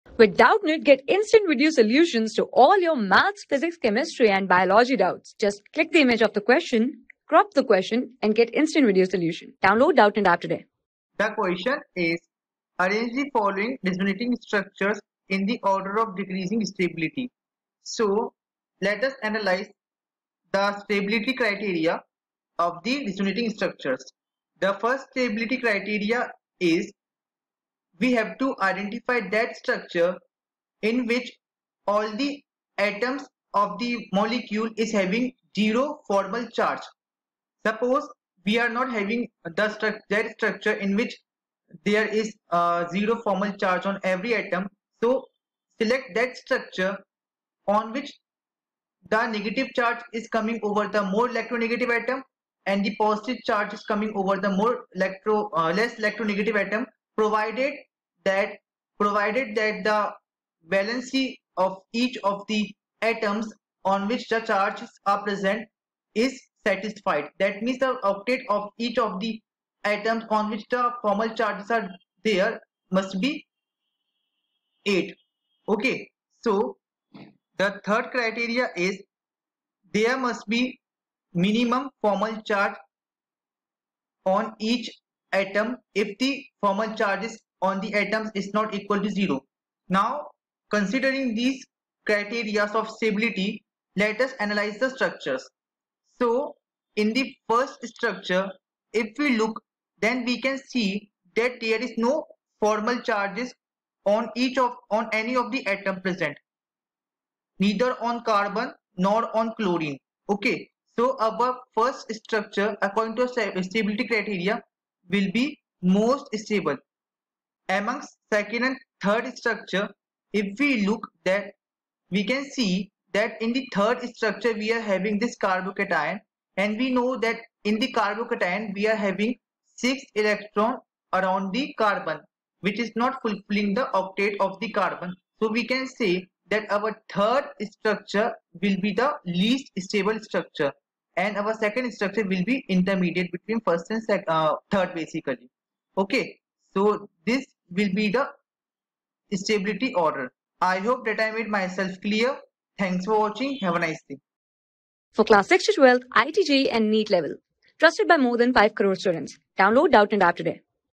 Arrange the following resonating structures in the order of decreasing stability :underset((Viny...
Arrange the following resonating structures in the order of decreasing stability :underset((Vinyl chloride))underset((I))(CH_(2)=CH-ddotunderset(..)CI: )harrunderset((II))( :overset(Theta)(CH_(2))-CH=overset(o+)underset(..)CI: )harrunderset((III))(overset(o+)CH_(2))-CH=underset(..)ddotCI^(Theta)
Class: 11
Subject: CHEMISTRY
Chapter: GENERAL ORGANIC CHEMISTRY
Board:IIT JEE

👉 Have Any Query? Ask Us.